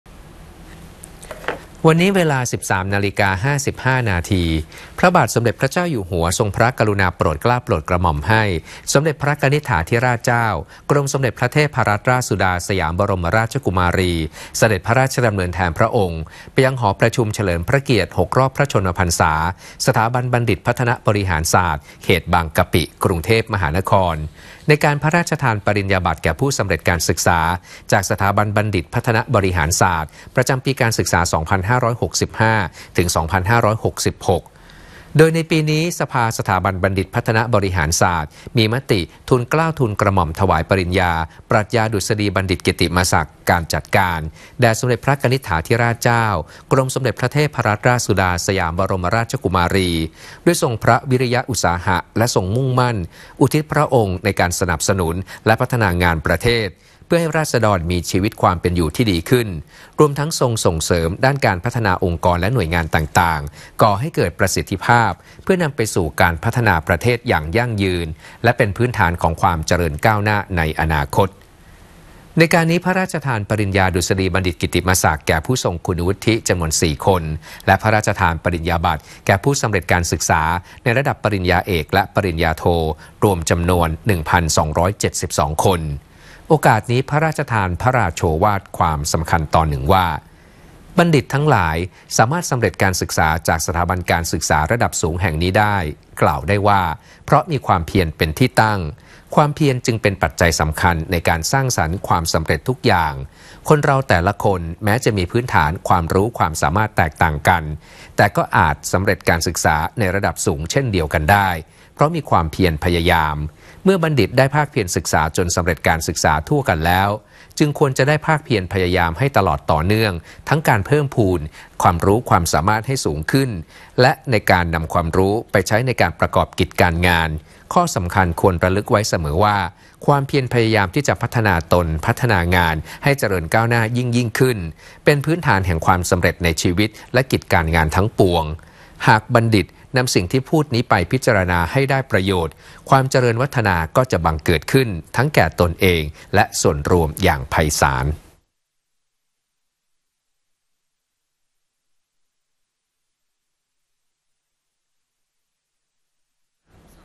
กรมสมเด็จพระเทพรัตนราชสุดาฯ เสด็จฯพระราชทานปริญญาบัตรสถาบันบัณฑิตพัฒนบริหารศาสตร์
วันนี้เวลา 13.55 น. พระบาทสมเด็จพระเจ้าอยู่หัว ทรงพระกรุณาโปรดเกล้าโปรดกระหม่อมให้ สมเด็จพระกนิษฐาธิราชเจ้า กรมสมเด็จพระเทพรัตนราชสุดาฯ สยามบรมราชกุมารี เสด็จพระราชดำเนินแทนพระองค์ไปยังหอประชุมเฉลิมพระเกียรติ 6 รอบ พระชนมพรรษา สถาบันบัณฑิตพัฒนบริหารศาสตร์ ในการพระราชทานปริญญาบัตรแก่ผู้สำเร็จการศึกษาฯ ประจำปีการศึกษา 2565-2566 ในการนี้ สภาสถาบันบัณฑิตพัฒนบริหารศาสตร์ มีมติขอพระราชทาน ทูลเกล้าทูลกระหม่อมถวายปริญญาปรัชญาดุษฎีบัณฑิตกิตติมศักดิ์ (การจัดการ) แด่สมเด็จพระกนิษฐาธิราชเจ้า กรมสมเด็จพระเทพรัตนราชสุดาฯ สยามบรมราชกุมารี ด้วยทรงพระวิริยะอุตสาหะ และทรงมุ่งมั่นอุทิศพระองค์ในการสนับสนุนและพัฒนางานประเทศ เพื่อให้ราษฎรไทยมีชีวิตความเป็นอยู่ที่ดีขึ้น รวมทั้งทรงส่งเสริมด้านการพัฒนาองค์กรและหน่วยงานต่าง ๆ ก่อให้เกิดประสิทธิภาพ เพื่อให้เป็นปัจจัยที่จะนำสู่การพัฒนาประเทศอย่างยั่งยืน และเป็นพื้นฐานของความเจริญก้าวหน้าในอนาคต จากนั้น พระราชทานปริญญาดุษฎีบัณฑิตกิตติมศักดิ์แก่ผู้ทรงคุณวุฒิ 4 คน และพระราชทานปริญญาบัตรแก่ผู้สำเร็จการศึกษาในระดับปริญญาเอก และปริญญาโท รวม 1,272 คน

โอกาสนี้ ได้พระราชทานพระราโชวาทแก่ผู้สำเร็จการศึกษาความว่า "บัณฑิตทั้งหลายสามารถสำเร็จการศึกษา จากสถาบันการศึกษาระดับสูงแห่งนี้ได้ กล่าวได้ว่า เพราะมีความเพียรเป็นที่ตั้ง ความเพียรจึงเป็นปัจจัยสำคัญ ในการสร้างสรรค์ความสำเร็จทุกอย่าง คนเราแต่ละคน แม้จะมีพื้นฐานความรู้ความสามารถแตกต่างกัน แต่ก็อาจสำเร็จการศึกษาในระดับสูงเช่นเดียวกันได้ เพราะมีความเพียรพยายาม บัณฑิตเมื่อได้พากเพียรศึกษาจนสำเร็จการศึกษาทั่วกันแล้ว จึงควรจะได้พากเพียรพยายามให้ตลอดต่อเนื่อง ทั้งในการเพิ่มพูนความรู้ความสามารถให้สูงขึ้น และในการนำความรู้ไปใช้ประกอบกิจการงาน ข้อสำคัญ ควรระลึกไว้เสมอว่า ความเพียรพยายามที่จะพัฒนาตนพัฒนางานให้เจริญก้าวหน้ายิ่ง ๆ ขึ้น เป็นพื้นฐานแห่งความสำเร็จในชีวิต และกิจการงานทั้งปวง หากบัณฑิตนำสิ่งที่พูดนี้ไปพิจารณาให้ได้ประโยชน์ ความเจริญวัฒนาก็จะบังเกิดขึ้นทั้งแก่ตนเองและส่วนรวมอย่างไพศาล"

ข่าวในพระราชสำนักประจำวันที่ 19 มกราคม 2567